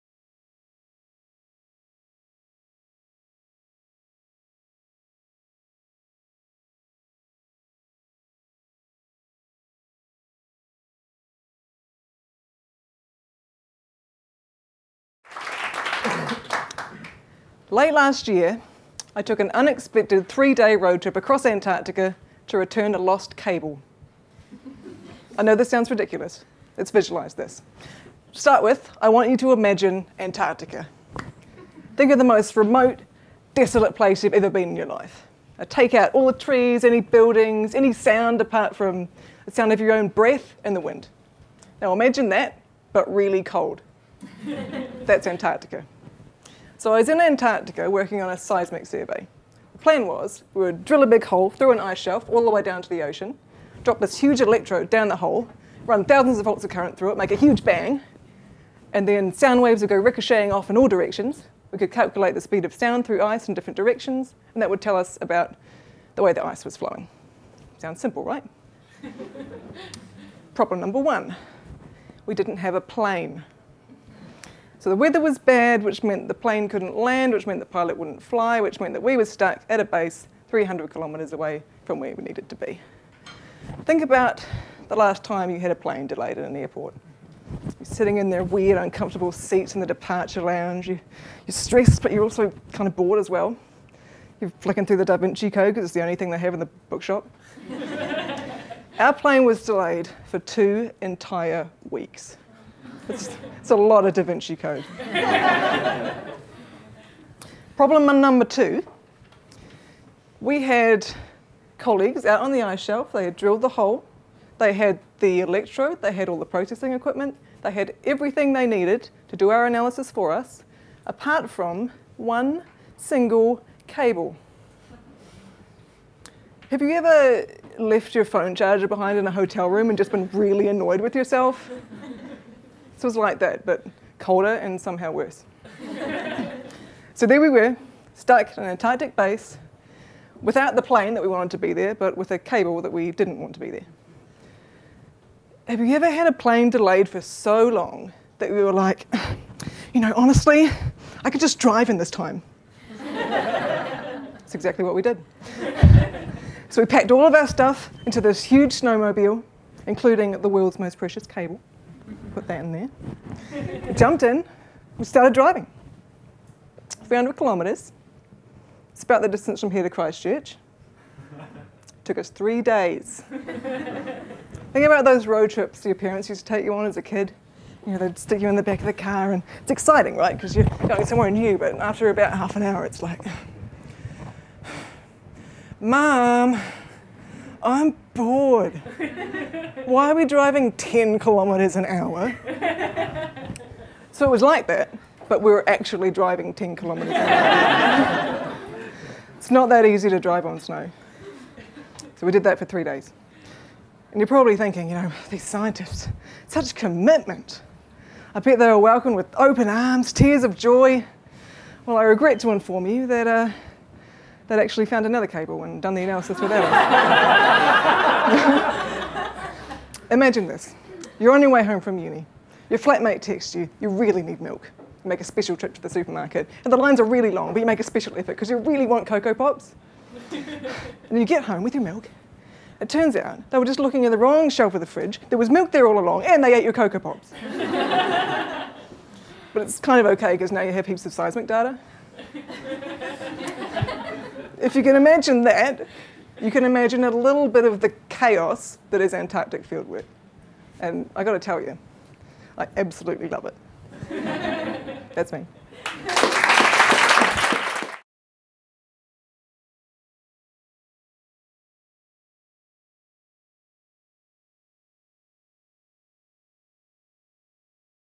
Go Beyond the Results - Lisa Craw
Lisa Craw's masters research in Geology is to investigate why Antarctic ice flows the way that it does. As part of her research she spent some time in Antarctica. She tells us the story of when a missing cable shows up somewhere it shouldn't be and the quest to retrieve it! Part of the Go Beyond the Results Thirst for Knowledge event during the NZ International Science Festival 2018.